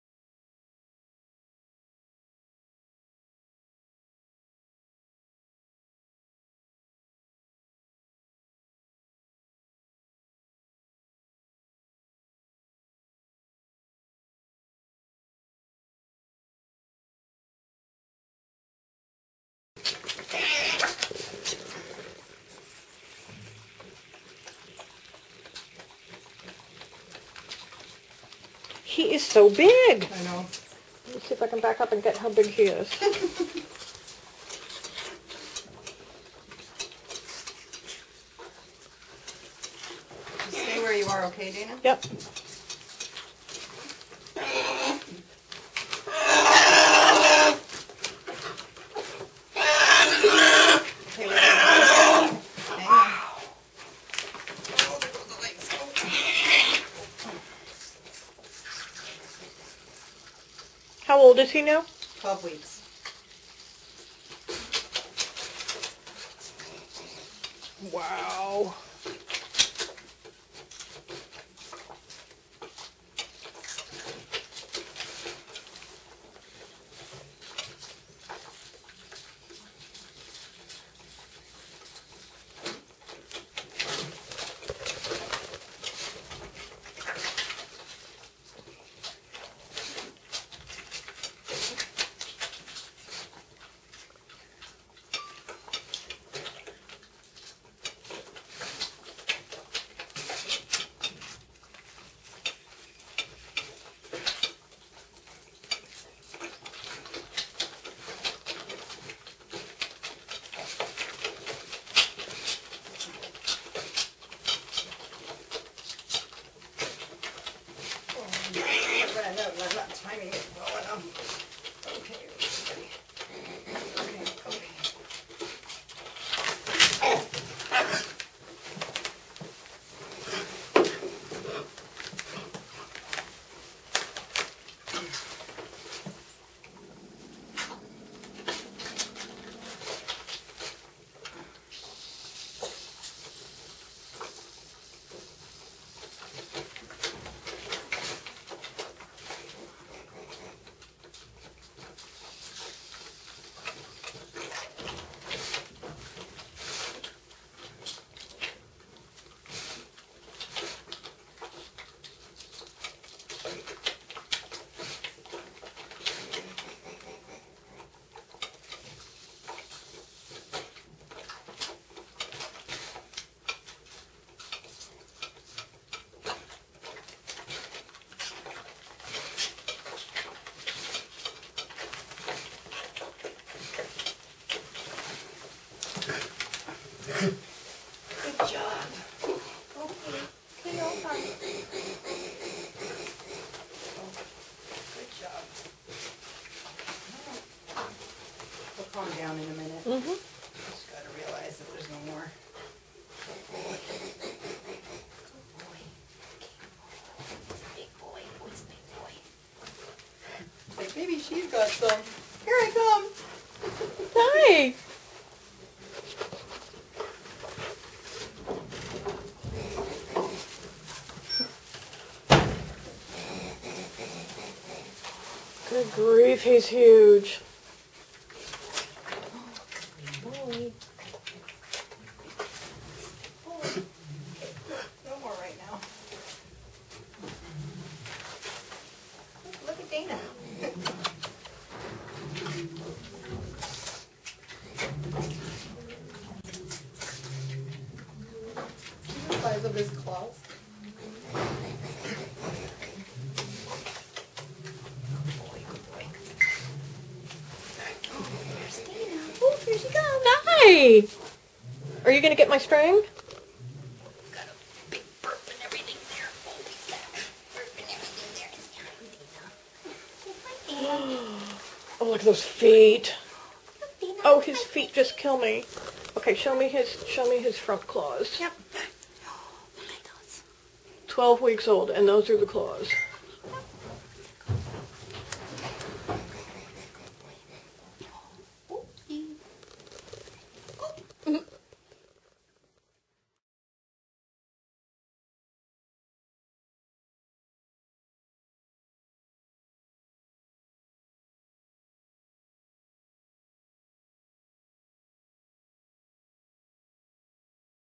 Bear Creek Sanctuary - Baloo Bear
12 week old Baloo eats breakfast. Fast.